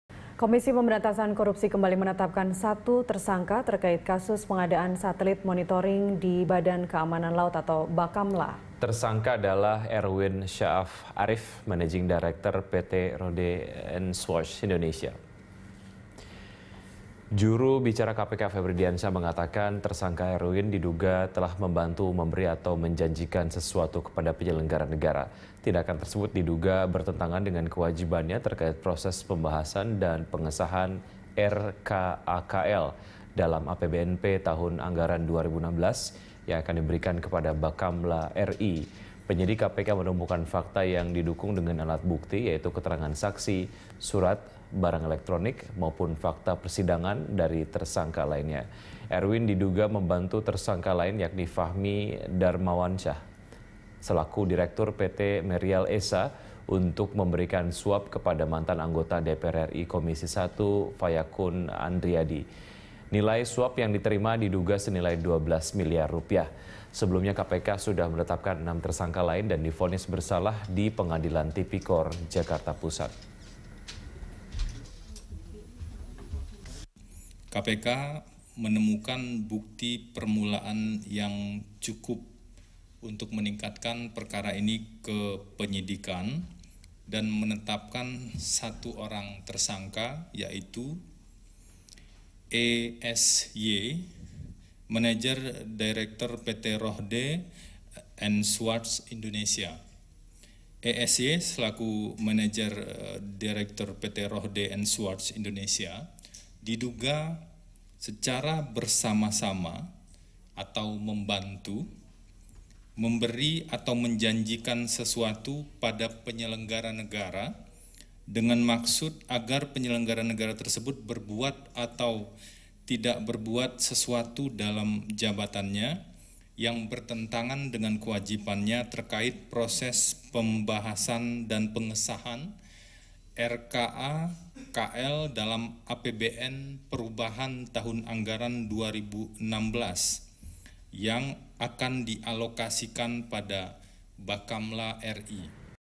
KPK Tetapkan Satu Tersangka Baru Kasus Bakamla
Komisi Pemeberantasan Korupsi (KPK) kembali menetapkan satu orang tersangka terkait kasus pengadaan satelit monitoring di Badan Keamanan Laut (Bakamla). Tersangka adalah Erwin Sya'af Arief, Managing Director PT Rohde and Schwarz Indonesia.

Facebook.com/BeritaSatu
Youtube.com/BeritaSatu